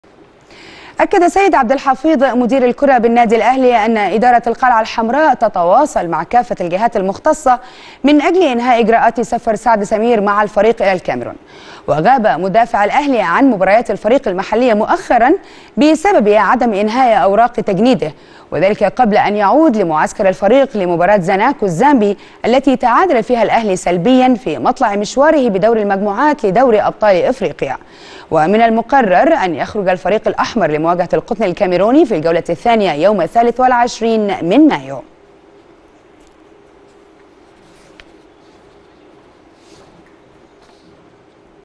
دوري أبطال إفريقيا.. الأهلي يحاول إنهاء إجراءات سفر سعد سمير مع الفريق للكاميرون
الموقع الرسمي للقناة  ..